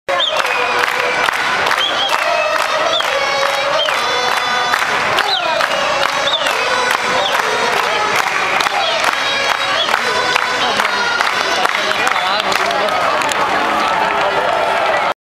03_Diada 2012, Avinguda Diagonal, Rambla Catalunya a les 17h
La Diagonal una hora abans de començar la manifestació dels 2 milions de catalans, la més gran del segle XXI a la península ibèrica, demanant "Catalunya un nou Estat d'Europa".